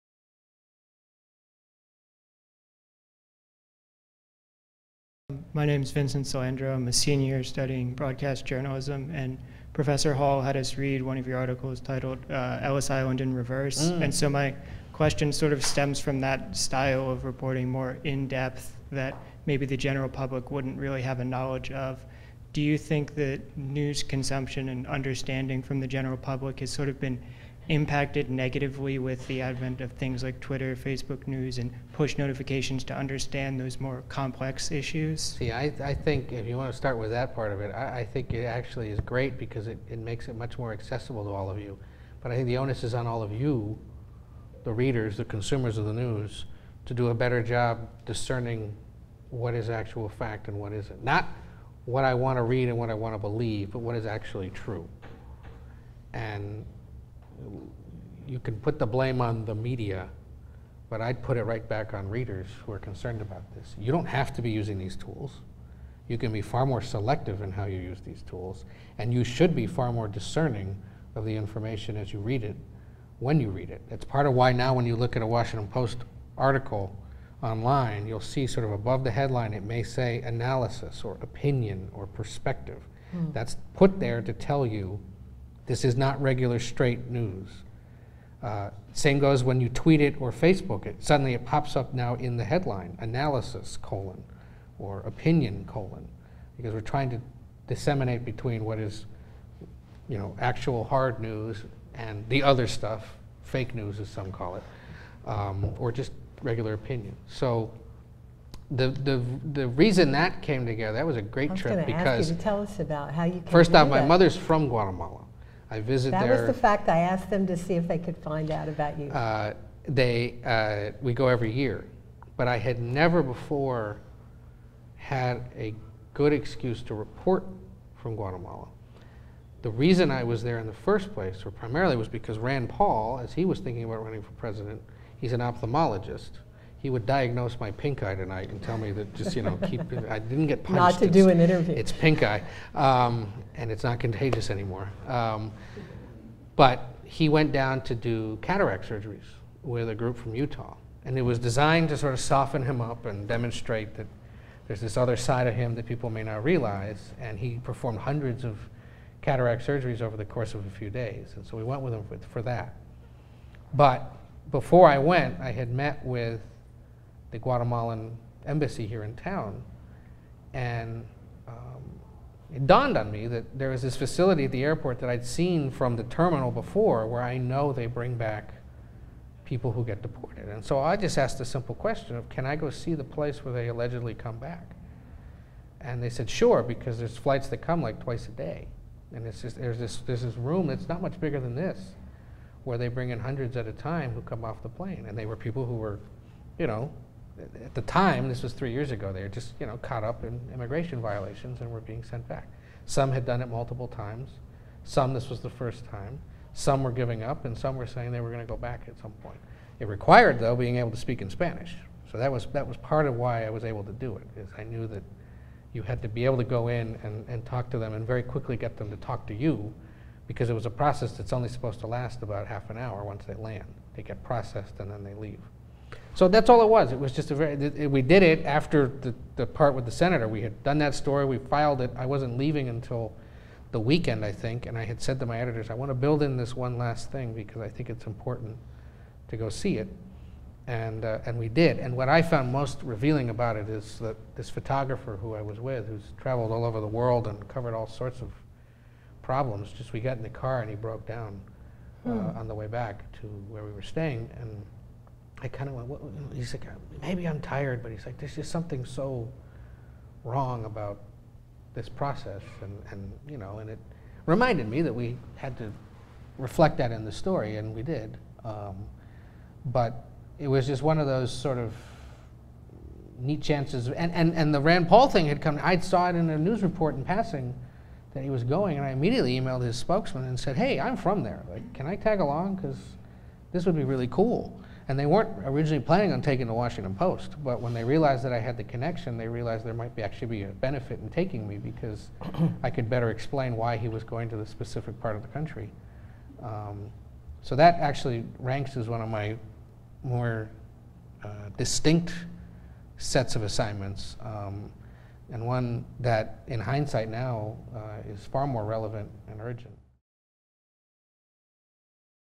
Ed O’Keefe on covering Sen. Rand Paul and deported immigrants in Guatemala
Washington Post reporter Ed O’Keefe shared an insider’s view on covering Congress and presidential politics in the Donald Trump era with students in the School of Communication at American University.  O’Keefe spoke to students in a special edition of the American Forum series from SOC that brings students together with prominent figures in media and politics. O’Keefe spoke with students about covering Sen. John McCain’s dramatic vote against a Republican effort to repeal and replace the Affordable Care Act known as Obamacare,  how members of Congress themselves feel about Congressional gridlock-- and how and why two New York politicians, Senate Minority Leader Charles E. Schumer and Donald Trump, are working together to protect “Dreamers,” some young people brought into the country illegally as children, from deportation since Trump ordered an end to the Obama-era Deferred Action for Children (DACA) program.

O’Keefe—who has been covering Congress and presidential campaigns since 2005—was on the campaign trail with Gov. Jeb Bush, Sen. Marco Rubio and the Republican field in the 2016 election and co-hosted eight hours of live election-night TV newscasts from the Washington Post.  O’Keefe spoke with future political strategists and future journalists in Prof. Jane Hall’s Politics and the Media course about covering Bush and Rubio and how reporting on Congress compares to covering a presidential campaign.  O’Keefe—whose mother was born in Guatemala—told students about returning there to report several stories.   Asked how his role as a journalist has changed since the election of Donald Trump, O’Keefe told students that the tumultuous election and current events “only reinforce” the need for explanatory journalism and analysis. O’Keefe is a 2005 graduate of the School of Public Affairs at American University, with a degree in political science.  He was a student in this inter-disciplinary Politics and Media class when he was an undergraduate.

In this segment O’Keefe talks about traveling to Guatemala to cover a trip by Sen. Rand Paul, an ophthalmologist, to perform eye surgeries for residents as a volunteer there.  He tells students about reporting on a second story, the return of Guatemalans deported from the U.S. as illegal immigrants.  He notes that this story is even more relevant today than when he wrote it several years ago—and he spoke about the value of “raising your hand” in the newsroom during events to report—objectively—on a story and a culture you know.